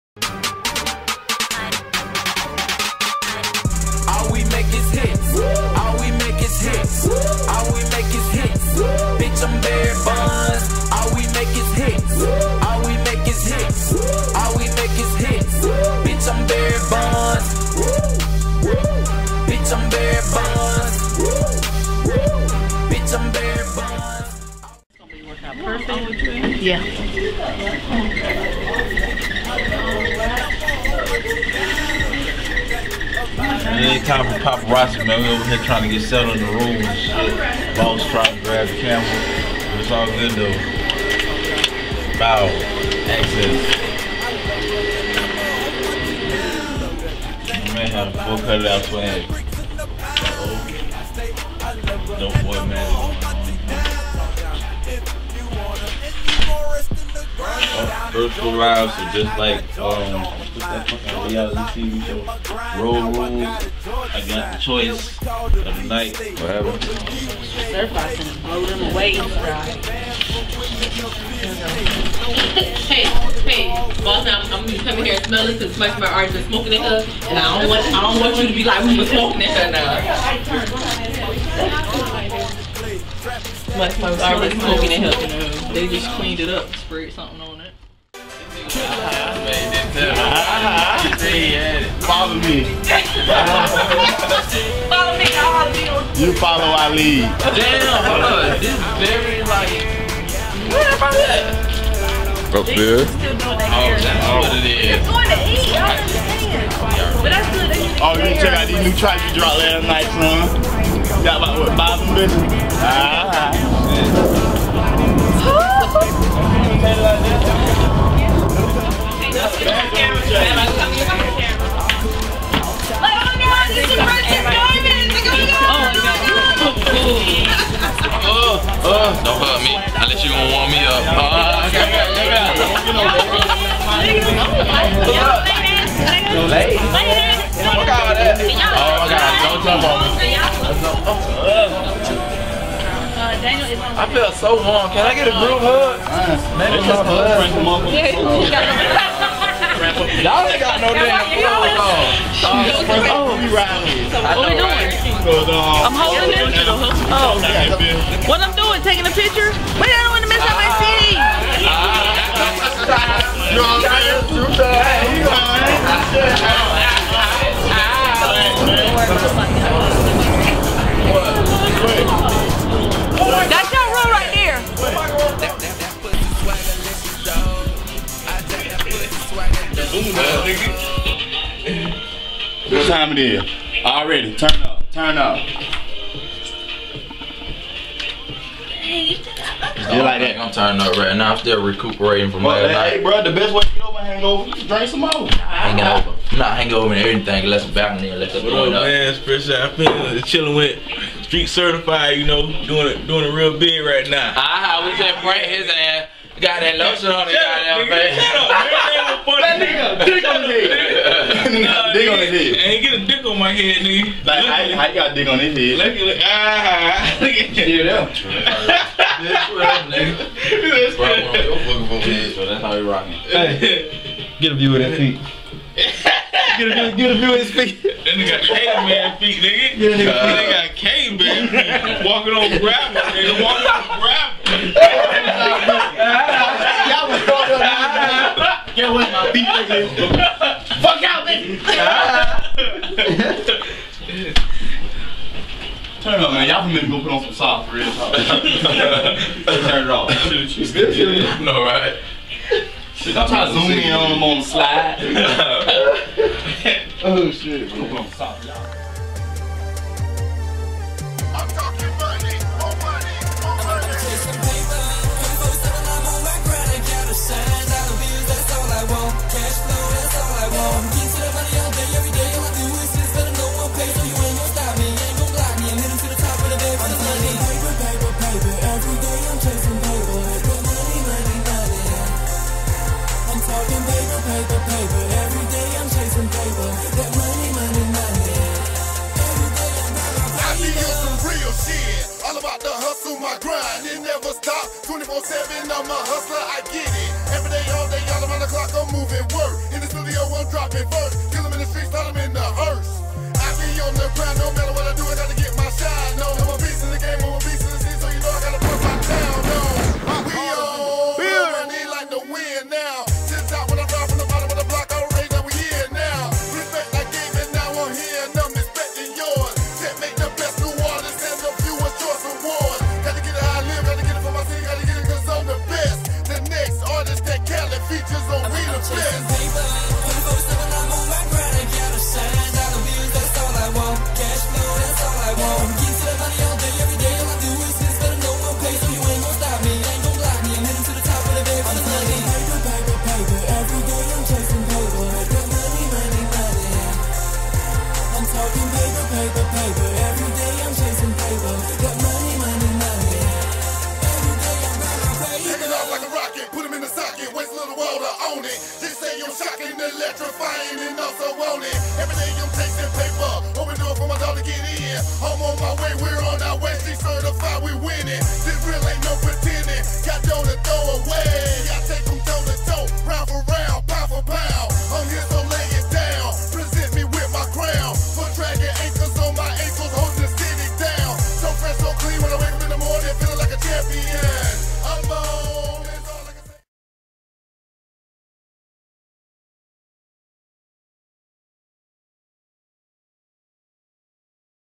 STREET CERTIFIED ENTERTAINMENT BEHIND THE SCENE
GO BEHIND THE SCENES WITH STREET CERTIFIED ENTERTAINMENT (OZEE, 380, CUT IT OUT SWAGG GANG, AND THE PRICELESS DIAMONDS) IN ATLANTA GA CHILLING BEFORE PERFORMANCES FOR ROCHARDER DJ T-ROC'S BIRTHDAY BASH!!!